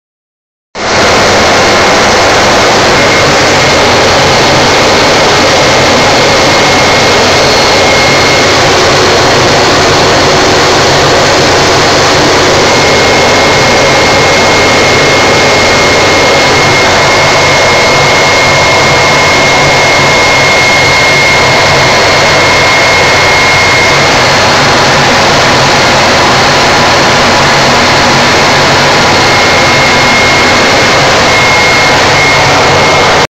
AV-8B NKT RTB 5
Sunrise takeoff Yuma, FL370 all day, quick fuel stops in Midland and Meridian, land at sunset Cherry Point.